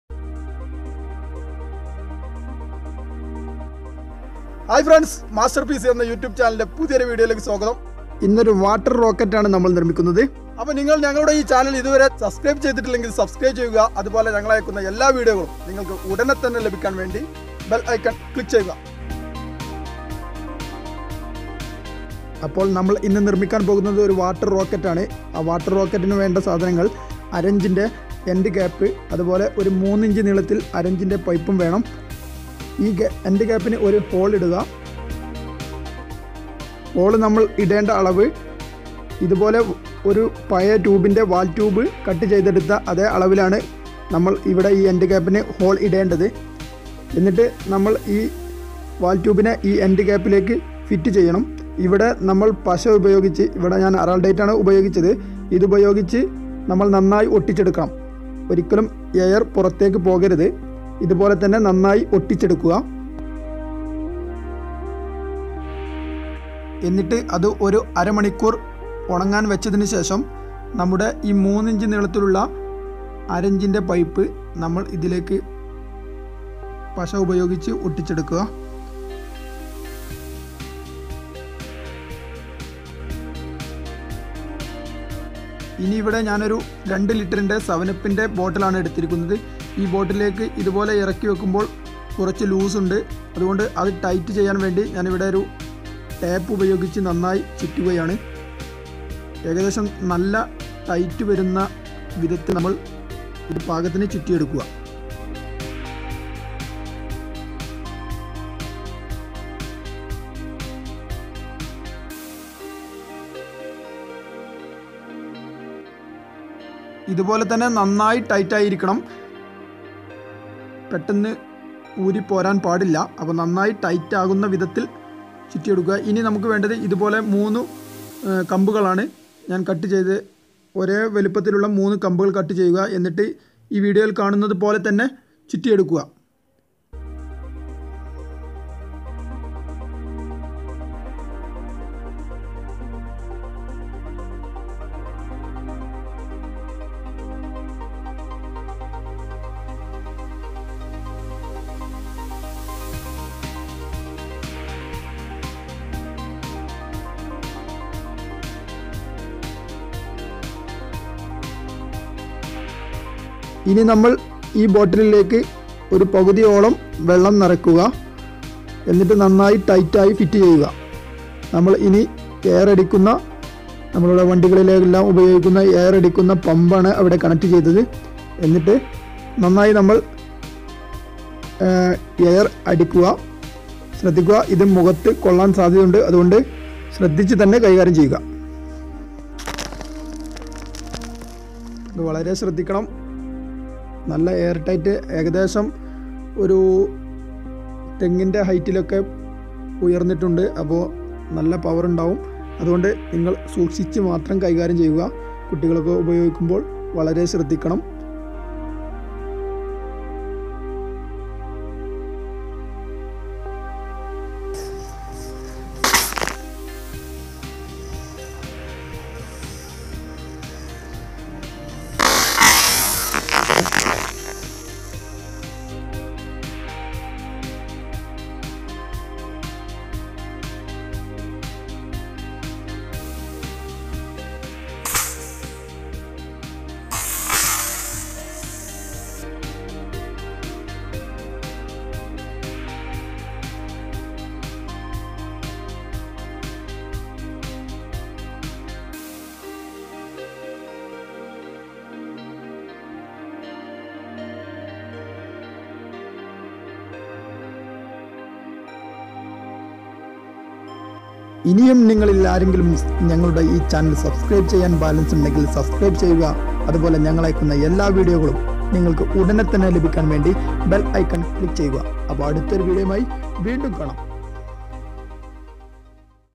Build A Simplest Water Rockets Launcher /കിടിലൻ "വാട്ടർ റോക്കറ്റ് "നിർമ്മിച്ചാലോ ??/
Build A Simplest Water Rockets Launcher

half inch pvc Pipe 3 inch hight
half inch pvc end cap
old Tube Valtube - 1
water bottle 2L